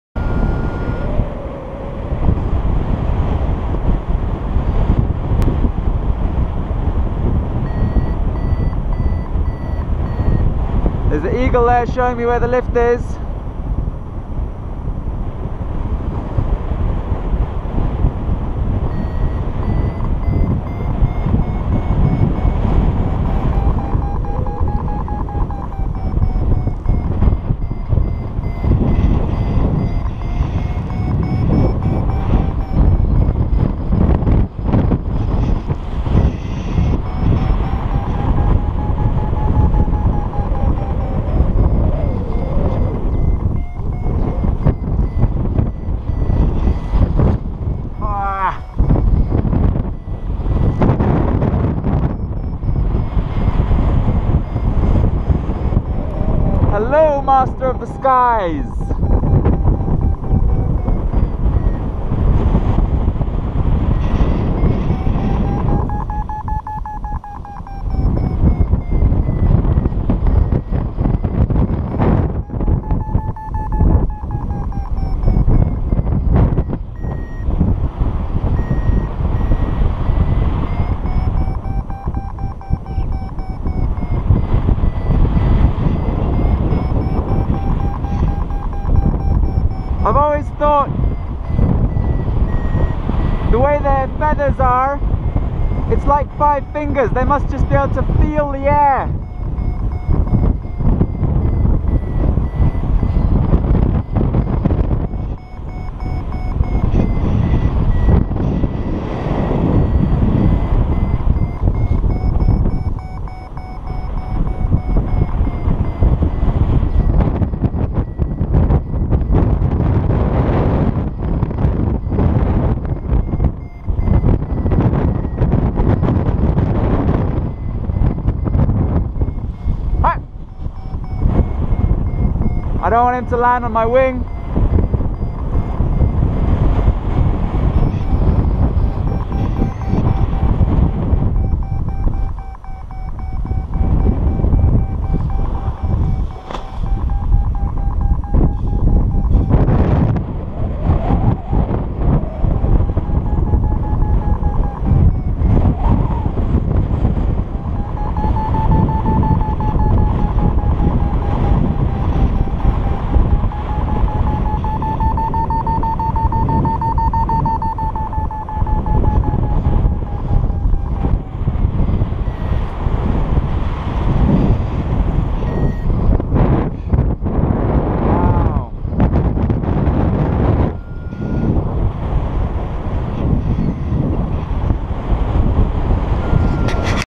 Paraglider flying with Eagles!
When free flying It can often be hard to know where thermals (rising air) are but seeing birds turning in tight circles is one of the best indicators.

This is one such instance. I was already flying towards the mountain to find a thermal under the big cloud and then I saw an eagle thermaling and i joined the climb. There is something amazing about sharing the air with birds like that...being so close. The gopro doesn't do this justice!